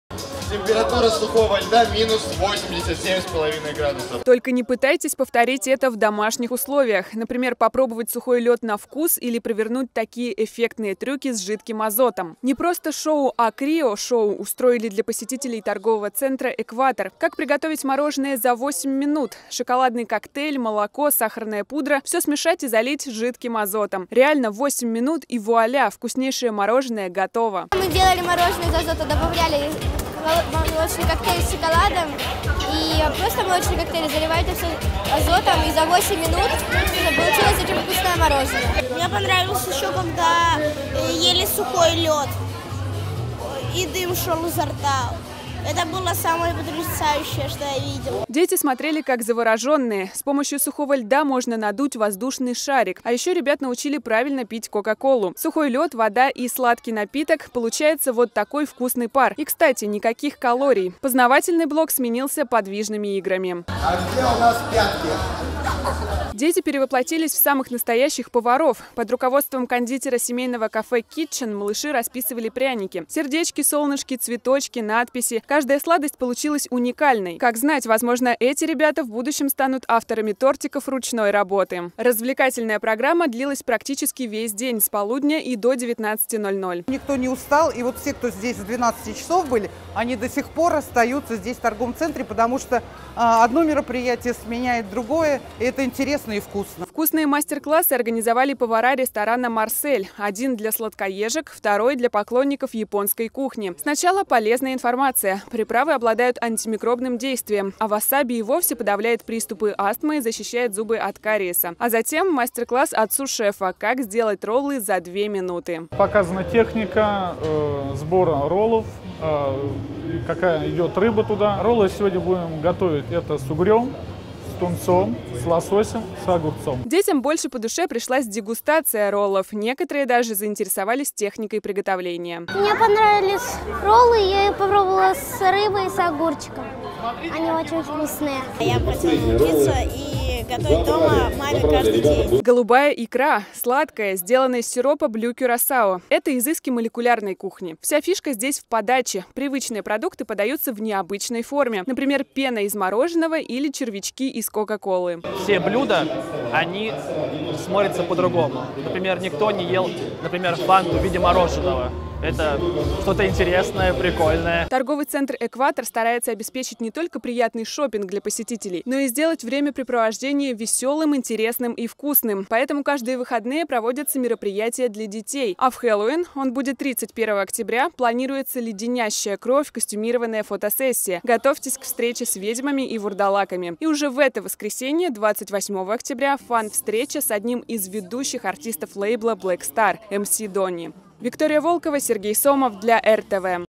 ГАСТРОНОМИЧЕСКИЙ ФЕСТИВАЛЬ 23.10.2018
Первый «Гастрономический фестиваль» прошел в Реутове. Участниками были исключительно дети. Несколько десятков человек учились, например, готовить сладости, расписывать пряники и делать роллы. Кроме этого все узнали, почему икра голубая, какова на вкус пена из мороженого и сорбет из фанты.